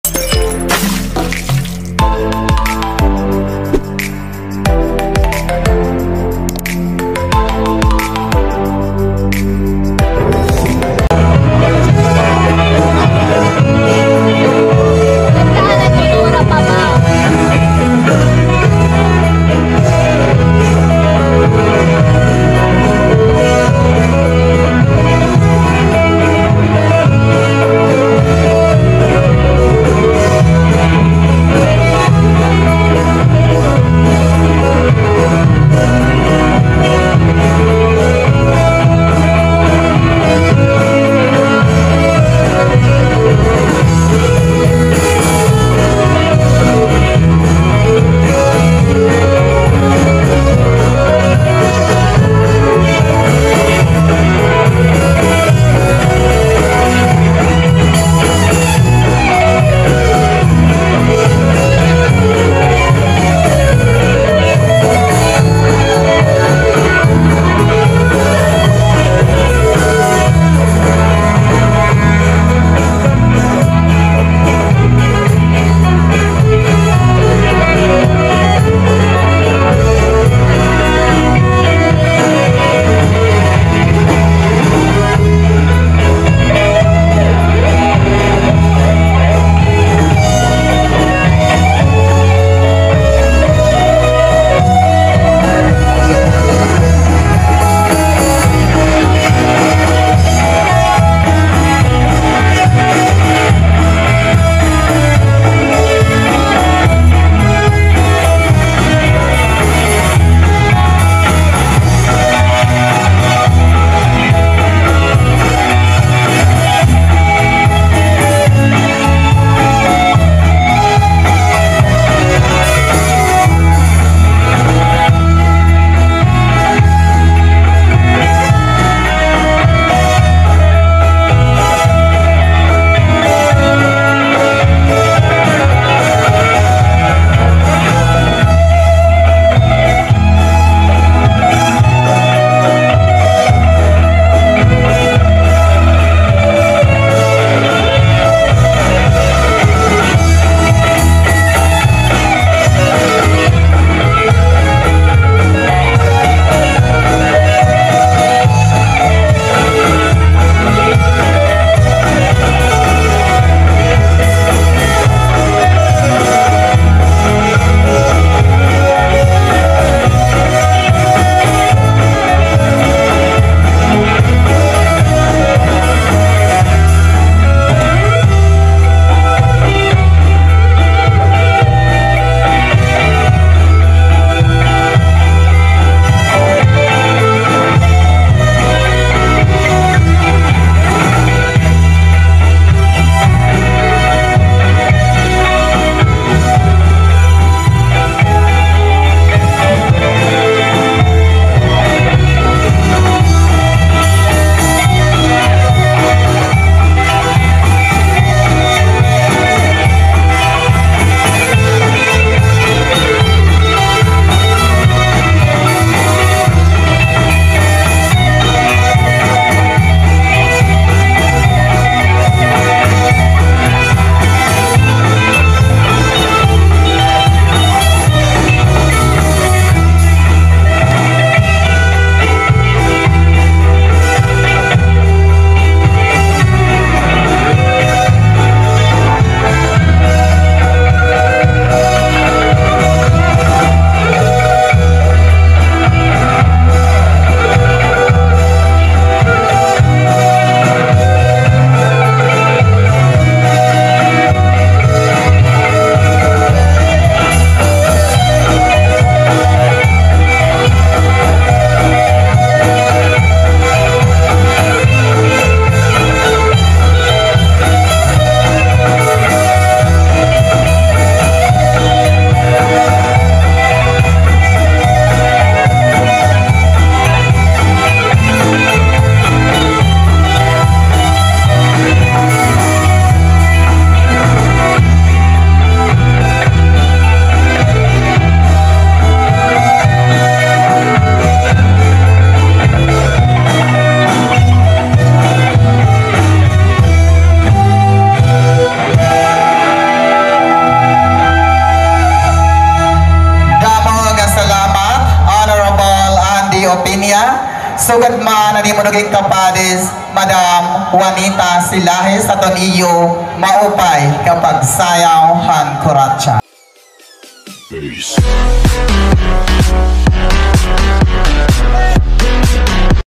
Kuratsa. SB Member Andy Opena & ma'am Juaneta Celajes Lapinig Tv
Good Evening mga idol.welcome again to my channel.my new uploaded video Kuratsa ng SB Member anday opena at Juanita celajes.i hope like again this video thank you and Gd blss ua all.